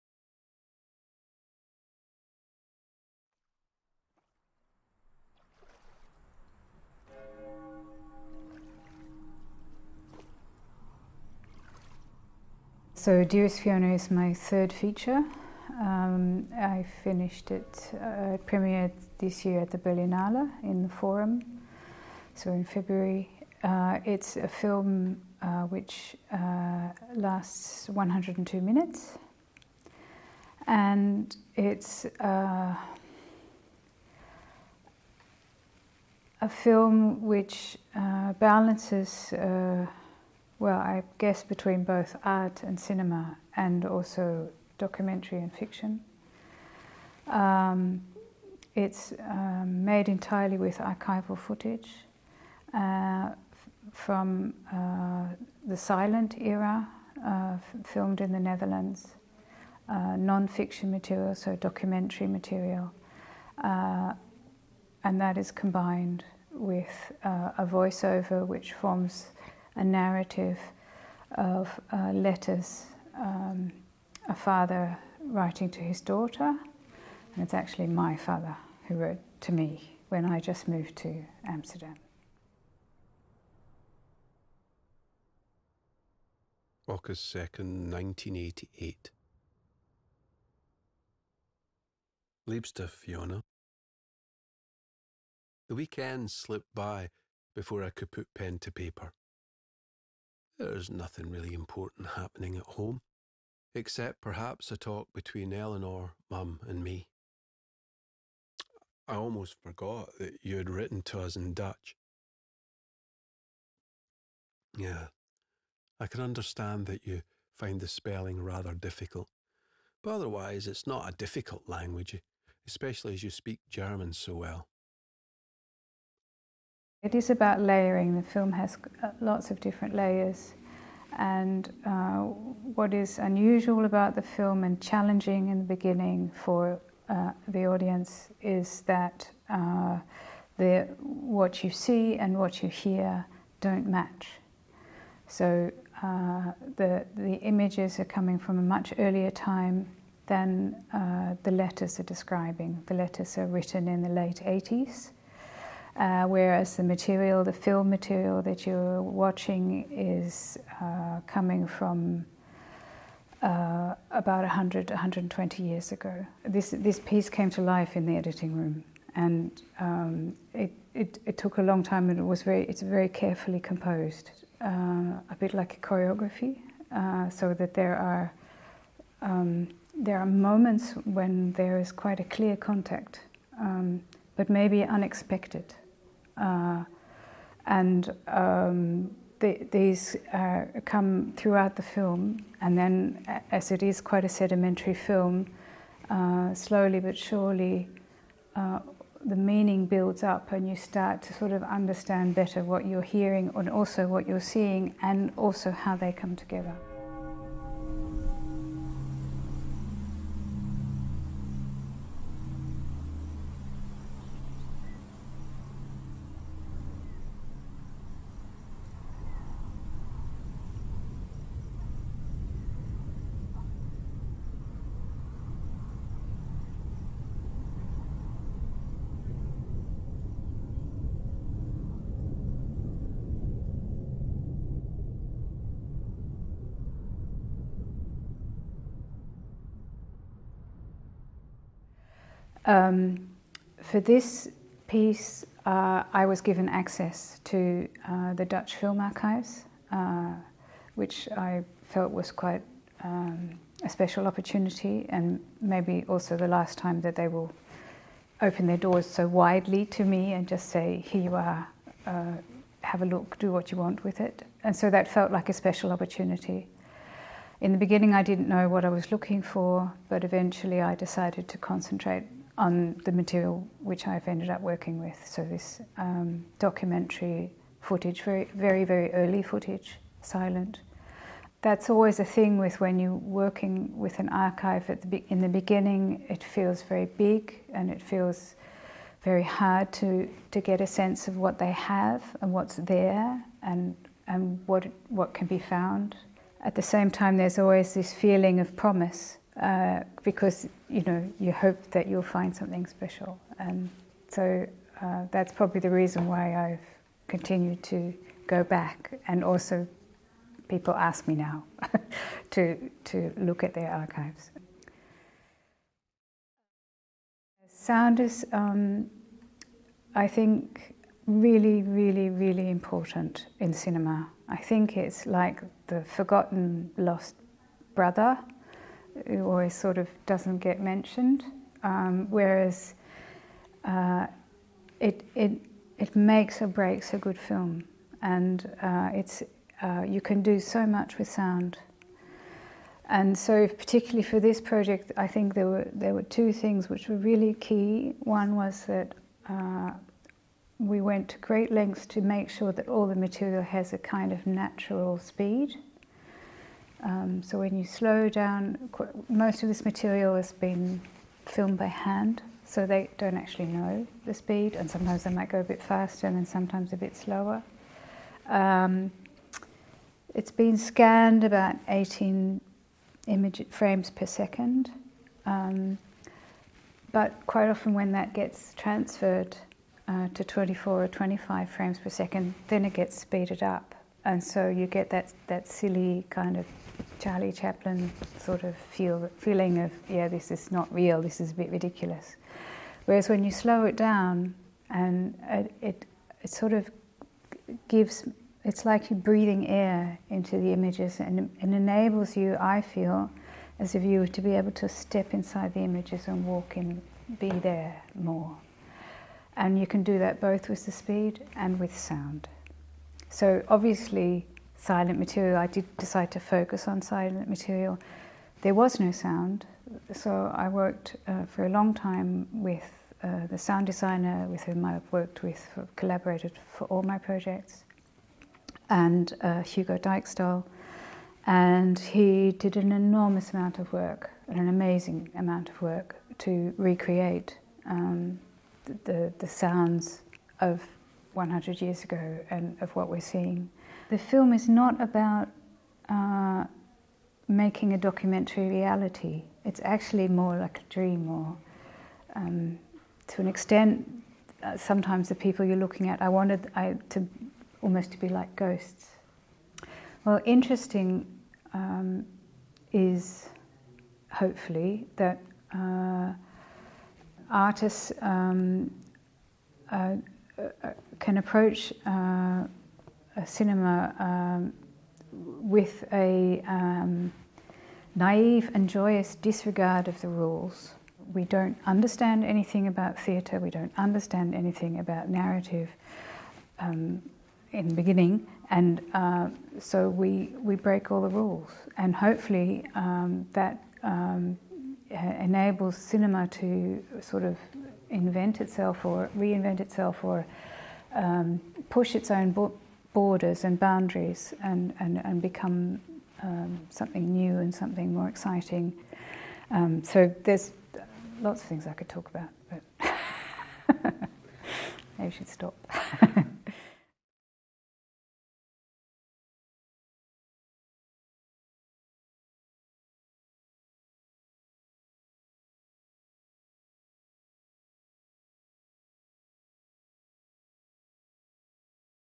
Fiona Tan Interview – Lo schermo dell'arte 2023
Recorded in Florence in November 2023  by Carolina Cappelli, on the occasion of the 16th Schermo dell’arte - Cinema and Contemporary Art Festival

Lo schermo dell’arte - 16° Edition
Florence, November 15 - 19, 2023